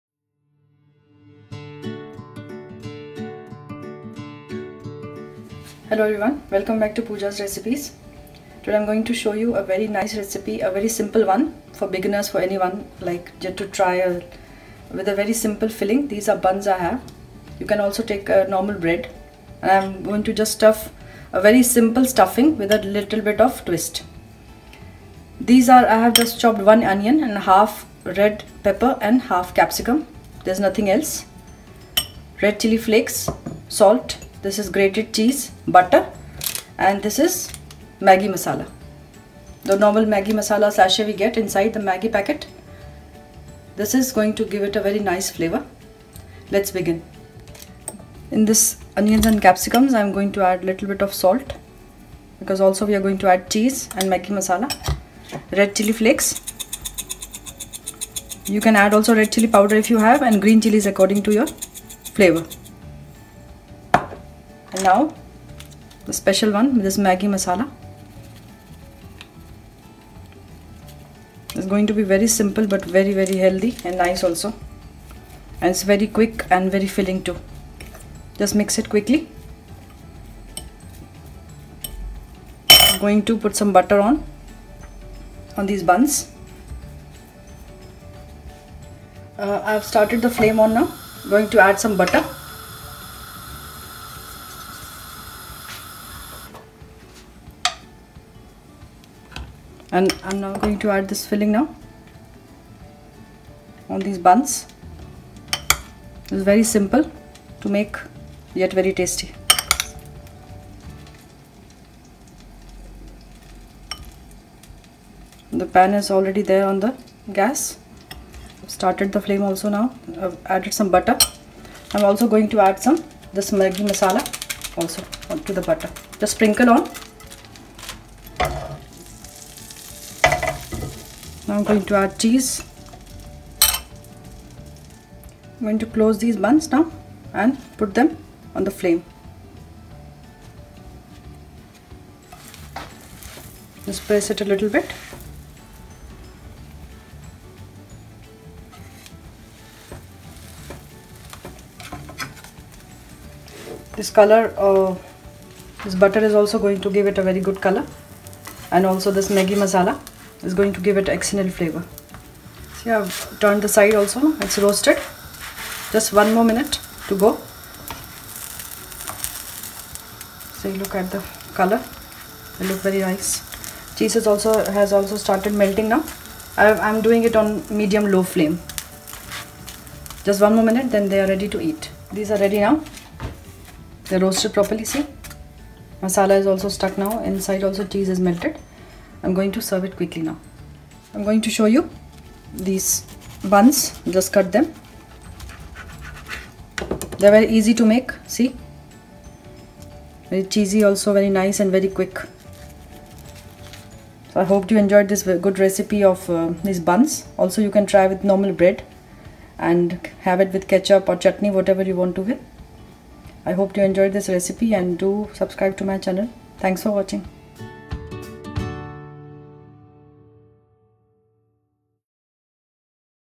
MAGGI MASALA MUNCH - GREAT FOR BEGINNERS!!
S.S.D.N

A very easy n quick recipe of Maggi masala munch for beginners as maggi is loved by everyone so this little twist given in these buns with maggi masala , veggies n cheese makes it a perfect snack at anytime ...

If you like this recipe, give it a thumbs up .👍🏻👍🏻

Ingredients :

2 Buns / 4 bread slices
1 small onion
1/2 red pepper
1/2 capsicum
1 Maggi masala sachet inside the maggi Pack
Cheese grated
Red Chilly flakes
Butter
Salty taste